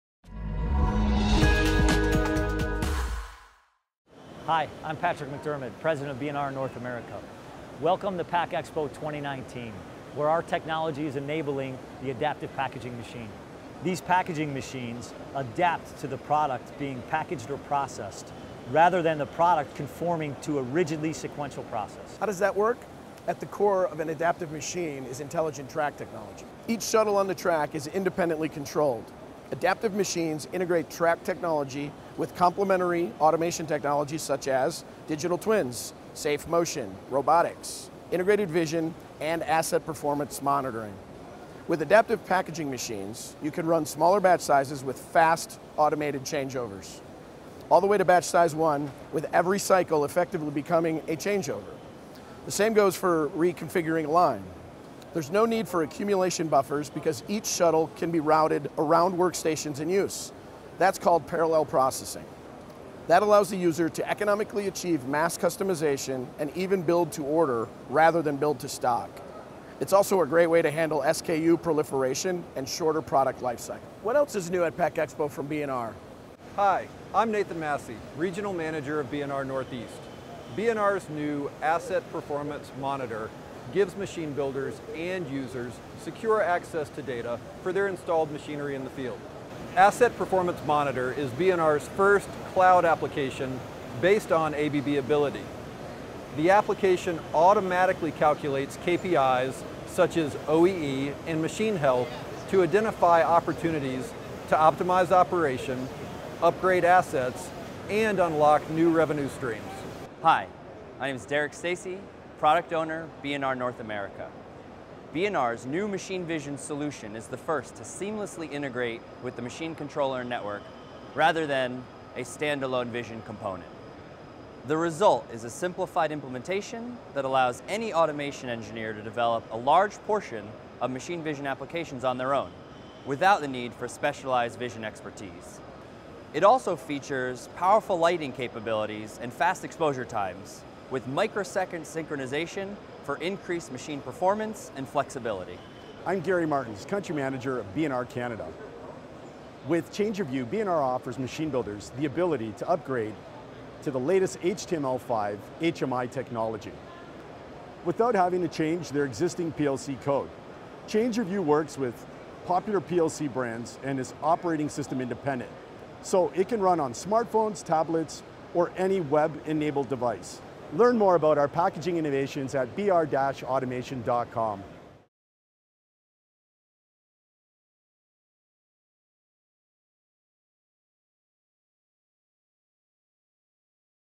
B&R's Packaging Innovations @ PACK EXPO 2019
B&R featured automation advances to maximize packaging performance and flexibility at PACKEXPO2019. B&R highlighted their ACOPOStrak intelligent track systems which enables what has become known as The AdaptiveMachine, representing the fourth generation of packaging machinery technology – allowing cost effective mass customization. B&R’s new machine vision solution integrates directly into the machine controller and network rather than a separate vision system and features a powerful lighting capability. Watch this video to learn more about the latest innovations shown at the PACK EXPO B&R booth.